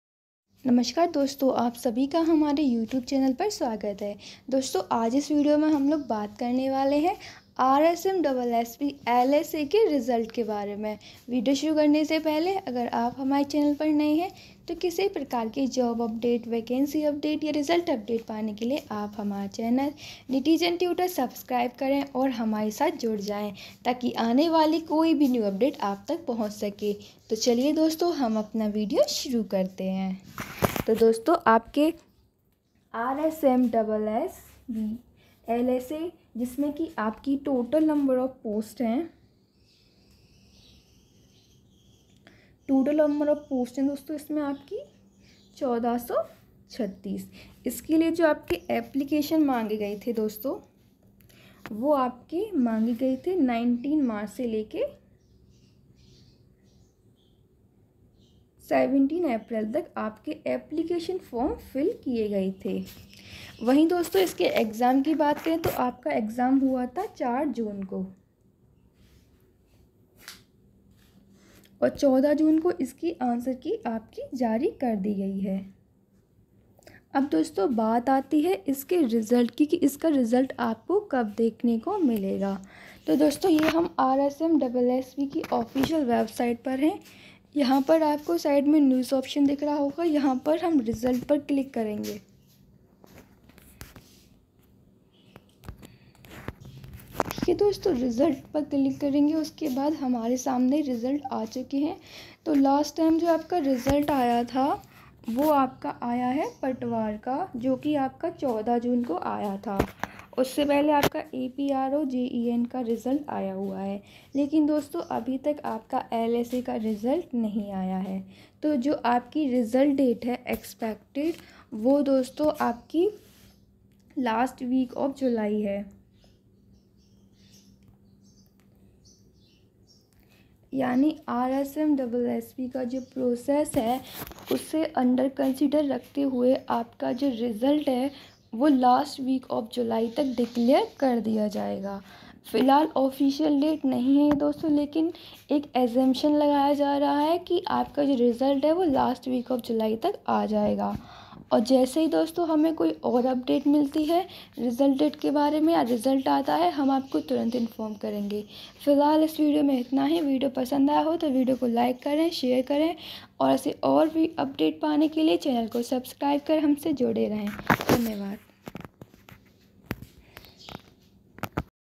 RSMSSB LSA Result Date | RSMSSB LSA Cutoff
About:-
In this video, I have informed about the new update regarding the declaration of result of RSMSSB LSA.